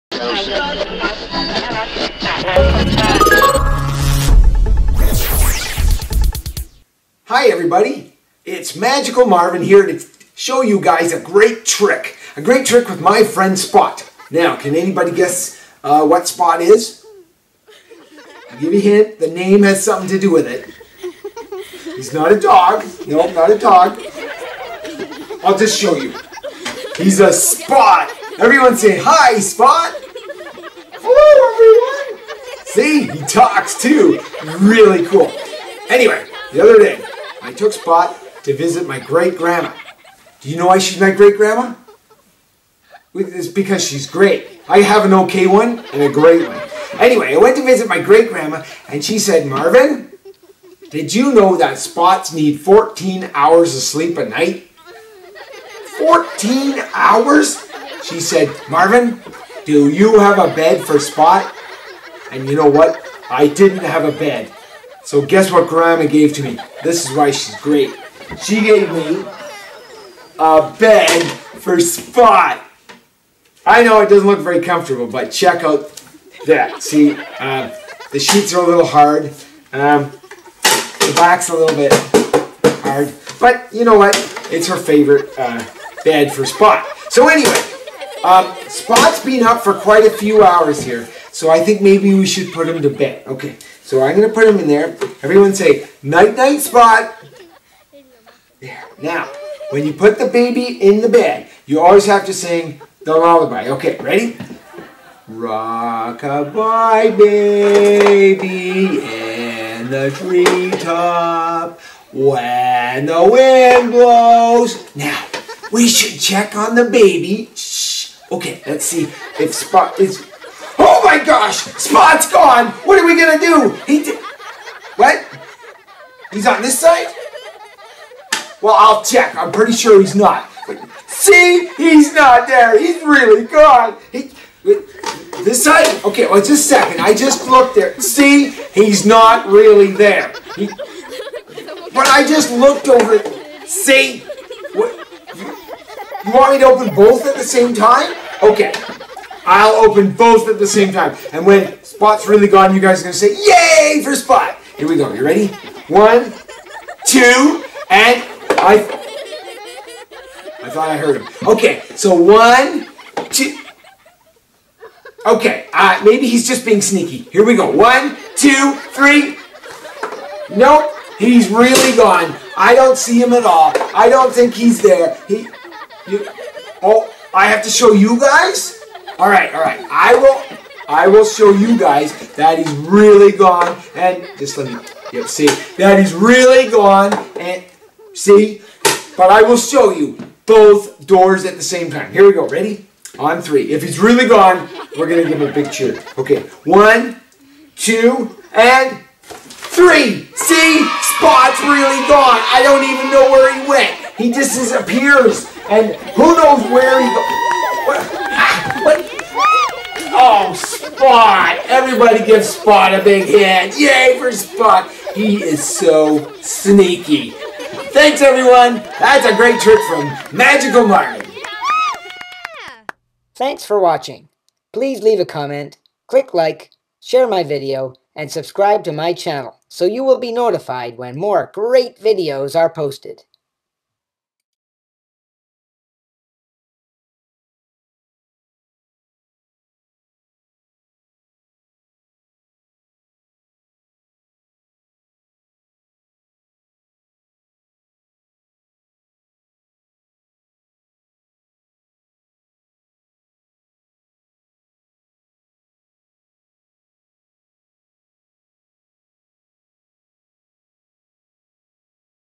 Ottawa Magician Magical Marvin's Hilarious Spot Trick
Ottawa Magician Magical Marvin performs his hilarious "Spot" trick!